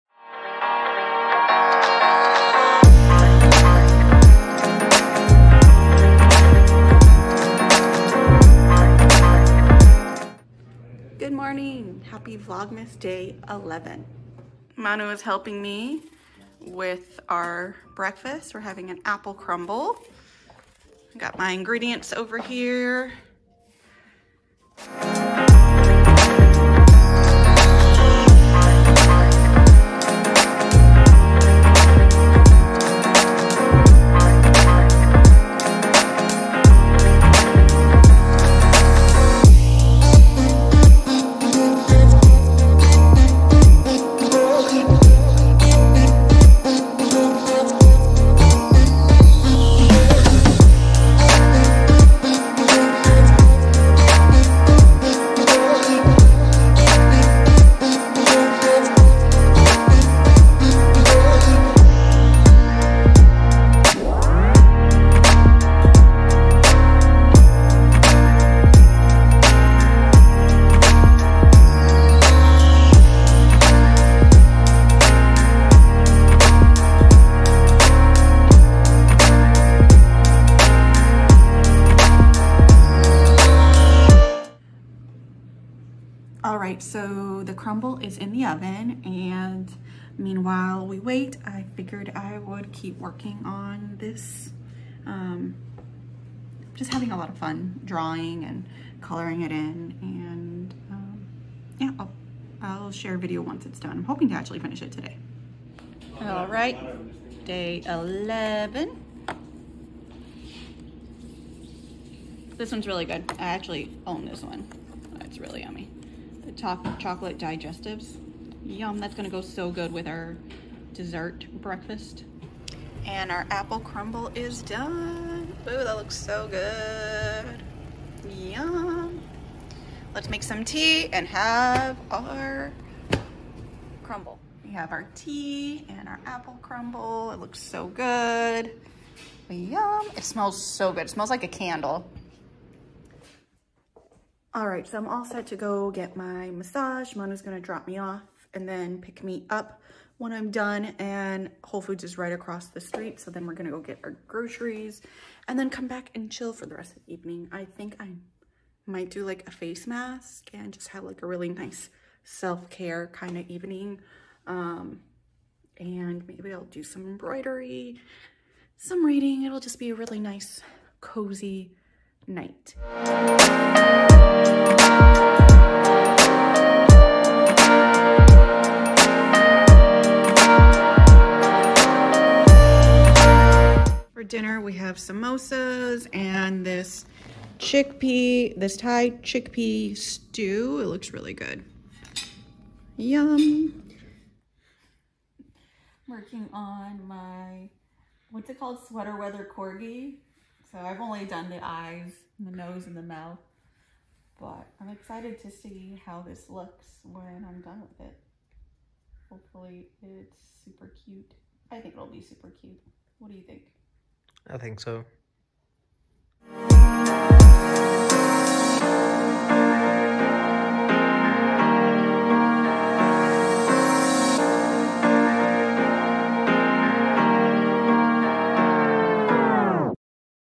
Vlogmas day 11- Holiday baking and cozy self-care day.
Vlogmas is here and I am so excited to share all the joy of the season with you. Shout out to @ChelseaCallahan for making relatable videos.